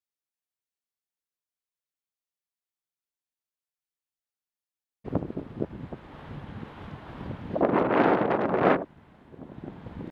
BV59UEK on the phone
It's a bit difficult to see since she was holding the phone quite high but you can see it between the spokes of the steering wheel. I guess her phone is more important than her child.

Tuesday 30th September 2014 approx 17:20